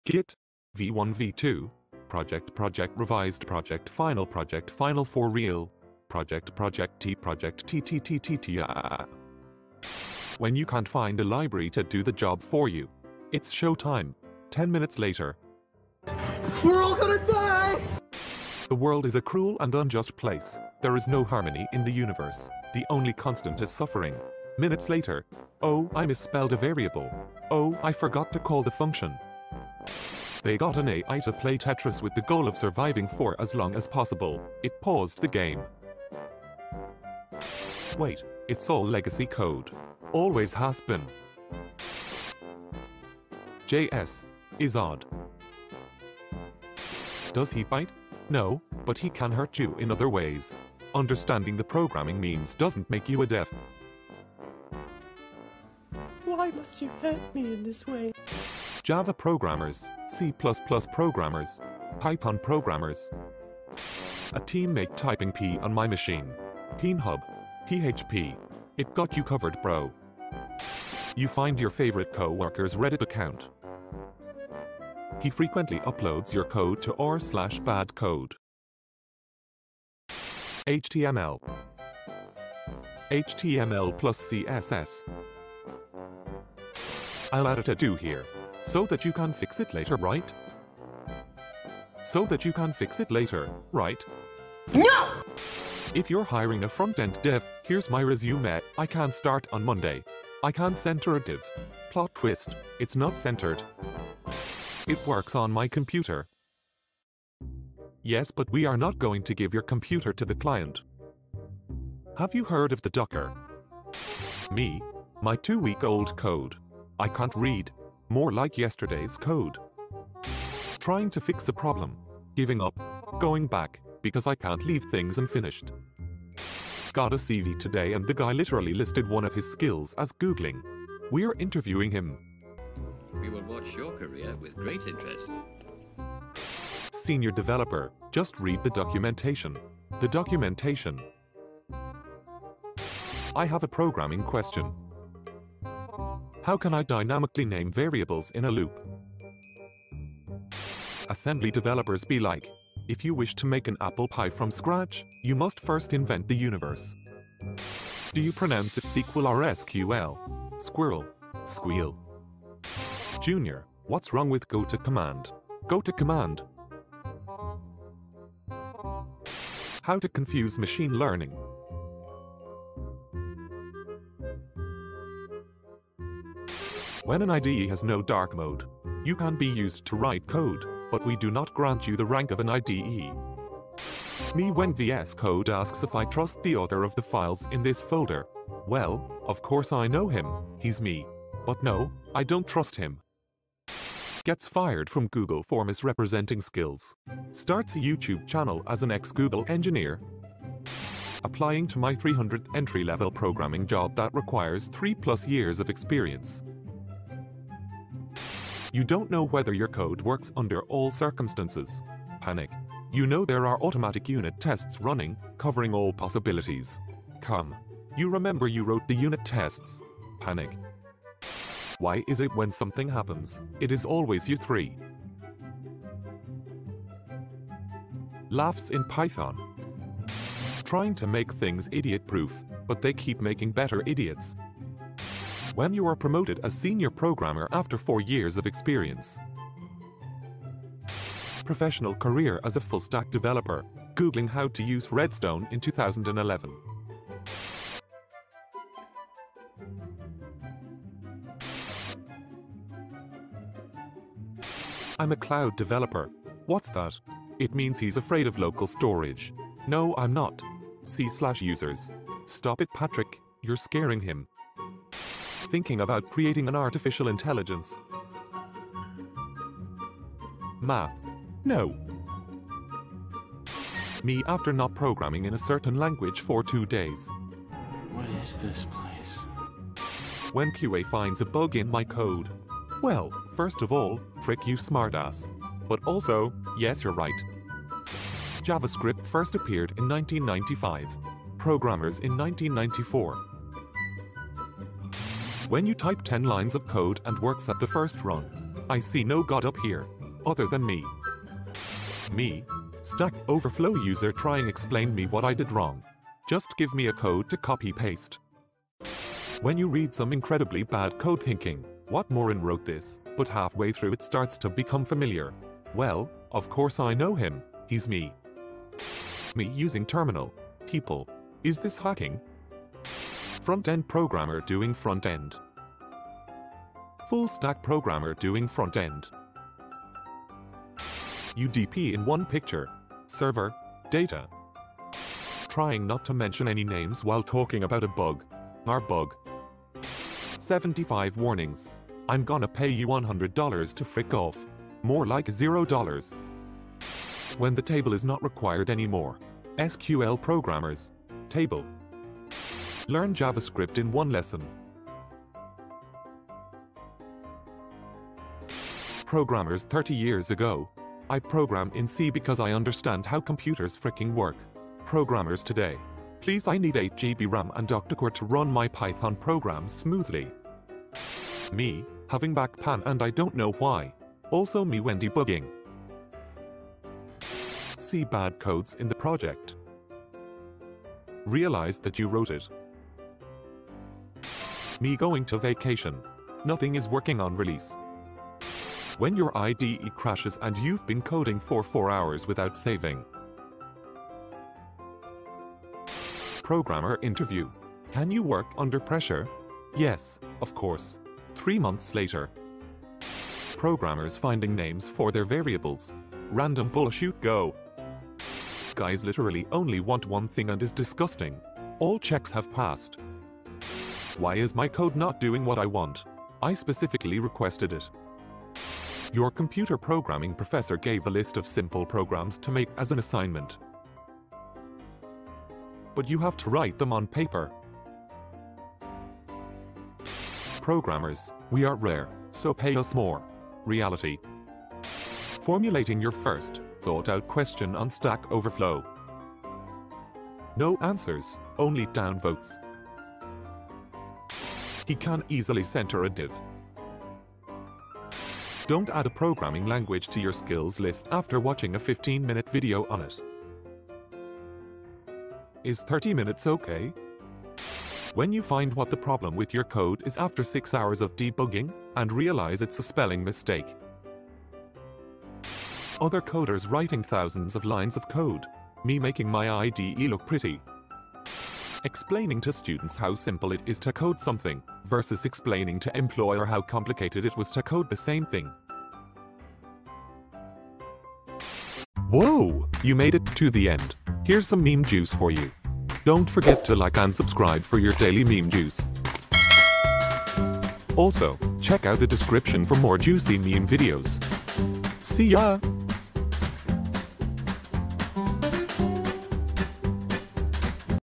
Funny Programming Memes || r/ProgrammerHumor 15
Let's check out some funny (or not?) programming memes from reddit r/ProgrammerHumor, if you're a nerdy programmer like me 🤓. Or maybe you just like programmer humor...